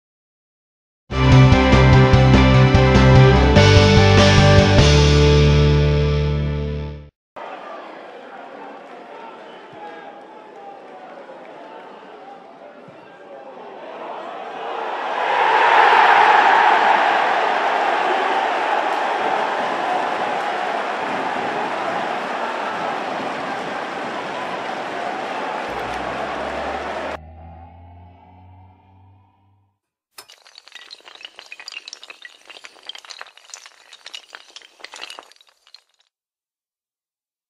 Peter Vincenti's Goal v Scunthorpe United: September 19th 2015
Rochdale AFC's Peter Vincenti netted the winner in Dale's 2-1 win over Scunthorpe United at Spotland.